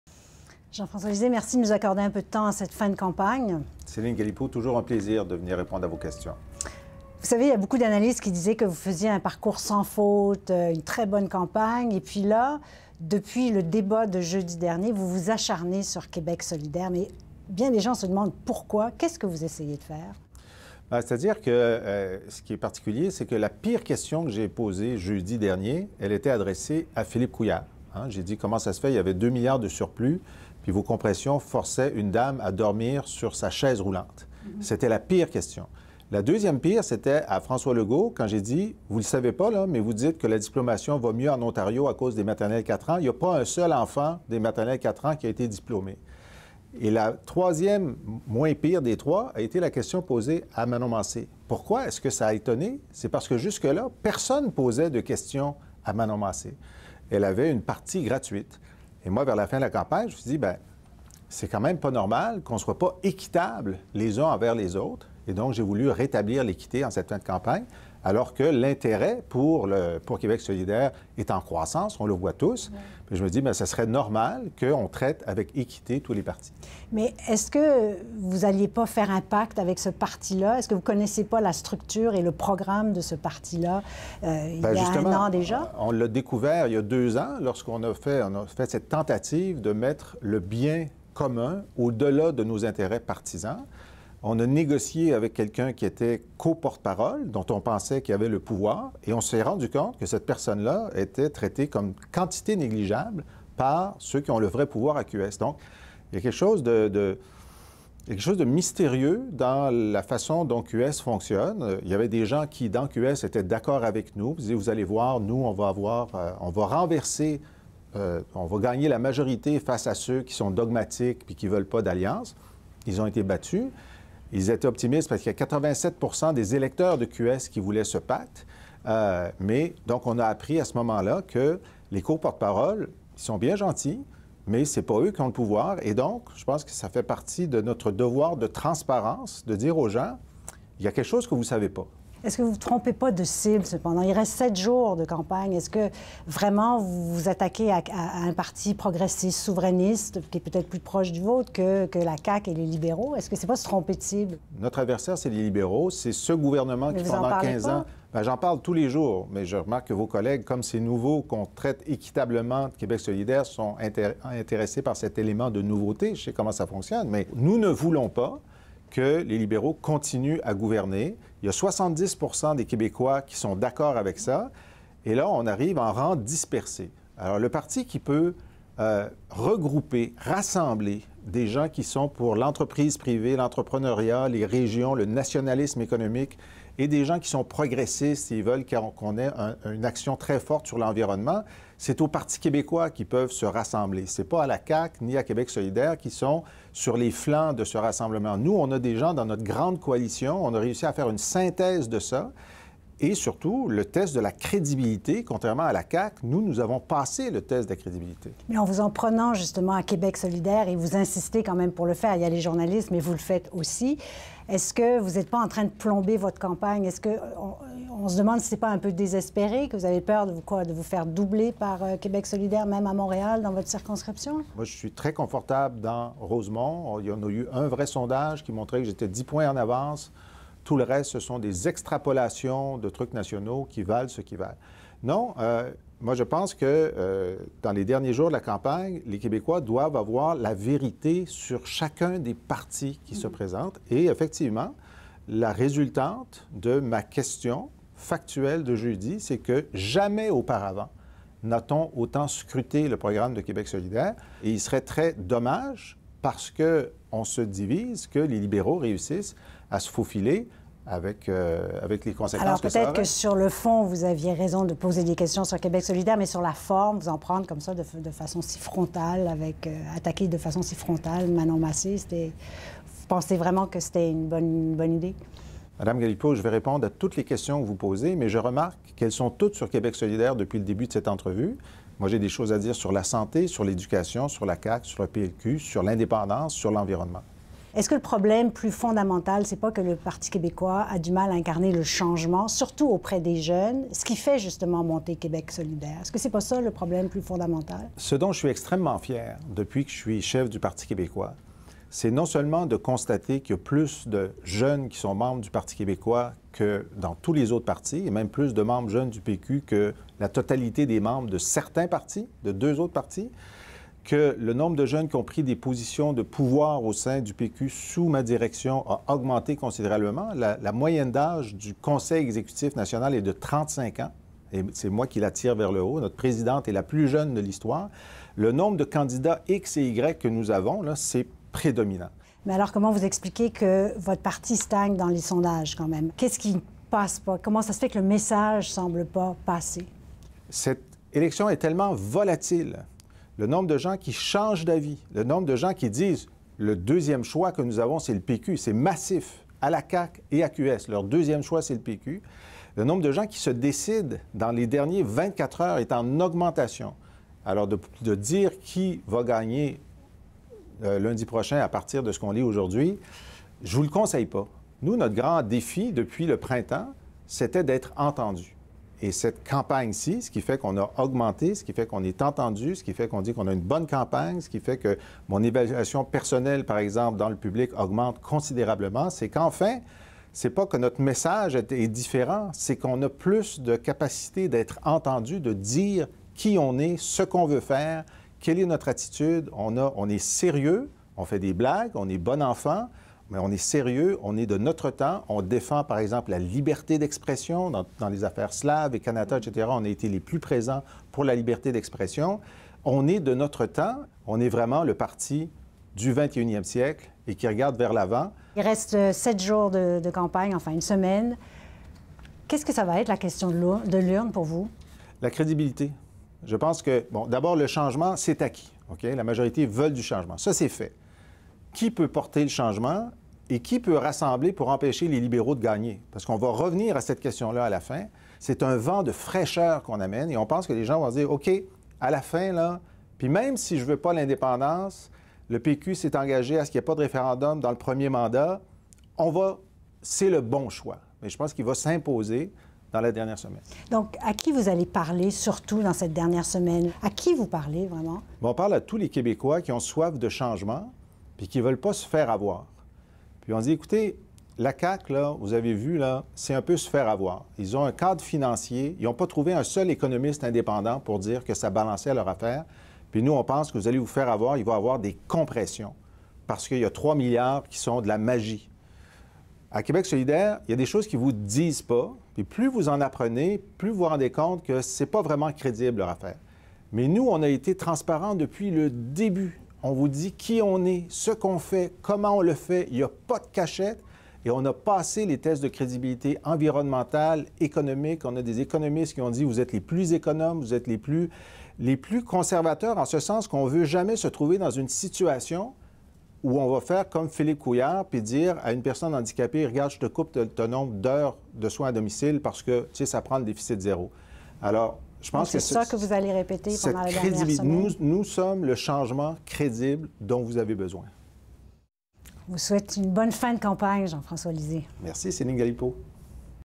Jean-François Lisée justifie ses attaques envers QS
Québec solidaire est devenu principale cible de Jean-François Lisée. À une semaine du scrutin, le chef du Parti québécois continue de croire que sa formation demeure la meilleure solution de rechange aux libéraux. Céline Galipeau s'est entretenue avec lui.